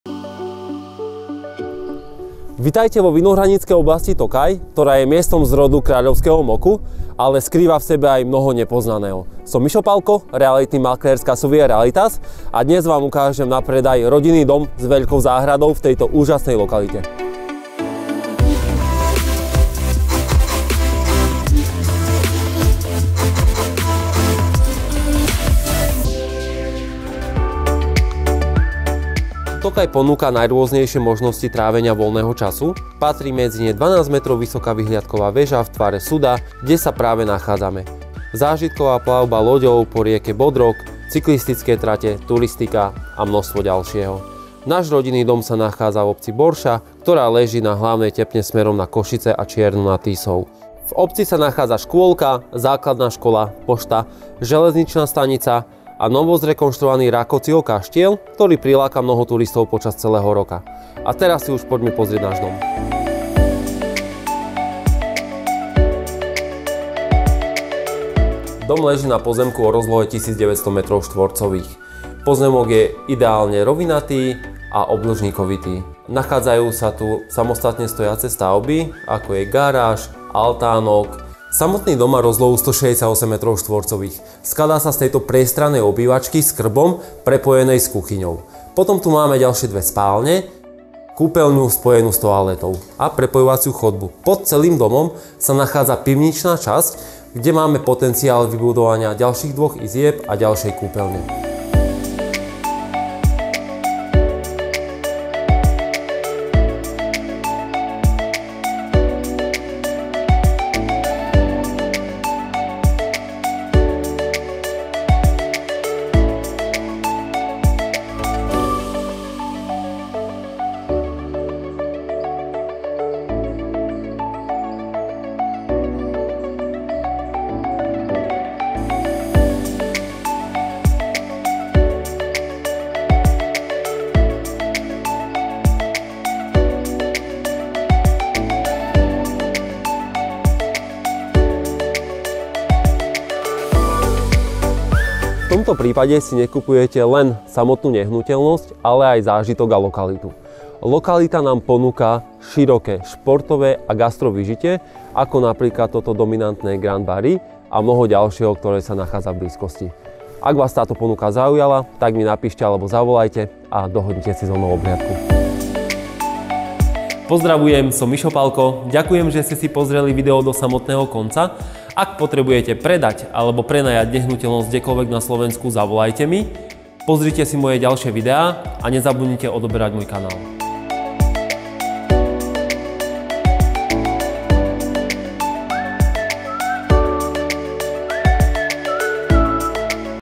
NA PREDAJ RODINNÝ DOM V OBCI BORŠA - TOKAJ - TREBIŠOV
MICHAL PALKO a realitná kancelária CASSOVIA REALITAS Vám ponúka na predaj rodinný dom v obci Borša v okrese Trebišov.

Obec je situovaná na hlavnej tepne smerom na Košice a Čiernu nad Tisou. Hranice s Maďarskom sú iba na kúsok. Do 20km máte termálne kúpalisko a hneď za hranicami zábavný park pre deti aj dospelých.

Dom je jednopodlažný a je kompletne podpivničený. Patrí k nemu balkón, samostatne stojaca garáž, drevený altánok. Pozemok je rovinatý s výmerou 1900m2.

Skladba domu : Základy domu sú betónové na štrkovom lôžku s vodotesnou izoláciou. Nosné murivo je prevažne so škvárobetónových tvárnic a tehál hrúbky 45cm. Krov je drevený sedlový s pôvodnou krytinou azbestovou. Okná sú plastové dvojsklo. Kotol v dome je plynový s termostatom a v dome sa nachádza krb s prieduchmi.

Prízemie domu tvorí zádverie a chodba, z ktorej sa dostanete do letnej kuchyne a izby, kt. sa využíva počas celého roka, pivničnej časti.

1.NP rodinného domu tvoria 2 dostatočne veľké izby, obývačka s krbom prepojená s kuchyňou, z ktorej je výstup na balkón, špajza a spojená kúpeľňa s toaletou.

Na pozemku pri dome je samostatne stojaca garáž, drevený altánok.

Pozemok je rovinatý a prispôsobený na pestovanie ovocných stromov aj zeleniny. Dom je napojený na obecnú kanalizáciu, elektrinu, obecnú vodu.

V obci je škôlka, základná škola, pošta, novozrekonšturovaný Rákocziho kaštieľ, ktorý láka mnoho turistov po celý rok. Tí, ktorí poznajú tento kraj, dobre vedia aj o skvelej vinárskej tradícií. Milovníci dobrého vína sa tu budú cítiť ako doma. Pre nového majiteľa kúpa rodinného domu je tou najlepšou investíciou. V budúcnosti ho môže využiť aj z pohľadu prenájmu, ktoré ponúka samotné okolie a Tokajská oblasť navštevovaná turistami.

Kúpna cena nehnuteľnosti je vrátane poplatkov súvisiacich s predajom (vypracovanie kúpnych zmlúv advokátom, poplatky za autorizáciu (overenie) podpisov predávajúceho, kolky za štandardné podanie návrhu na vklad kúpnych zmlúv do katastra prostredníctvom advokáta) a samozrejme kompletné právne poradenstvo a servis. Cena je vrátane provízie pre RK.

Cena tohto rodinného domu je 99.990,- EUR.

Ak vás táto ponuka zaujala tak nás neváhajte kontaktovať: Michal PALKO - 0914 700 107 - certifikovaný realitný maklér NARKS